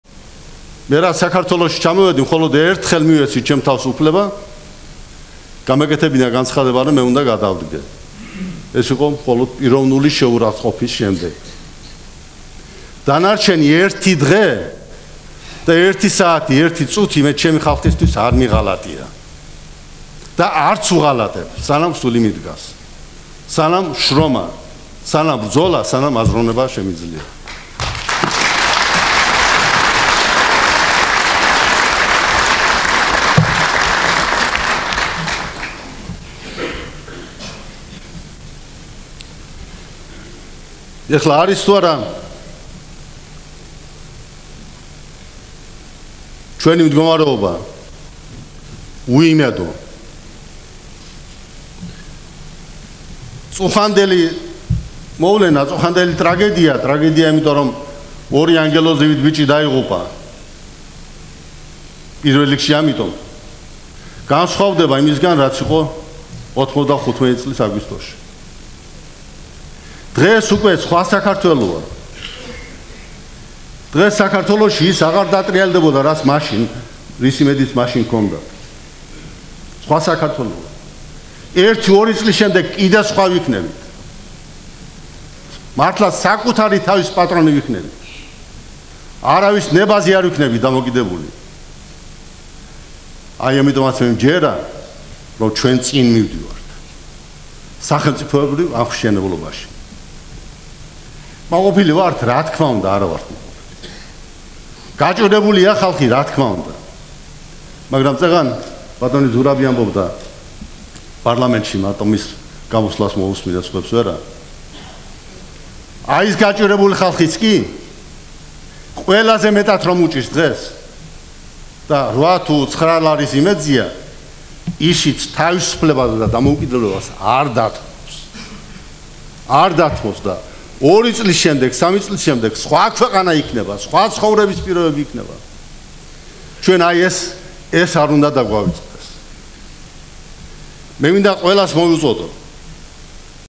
ე.შევარდნაძე:არ მიღალატია
1998 წ.მთავრობის სხდომის ფრაგმენტი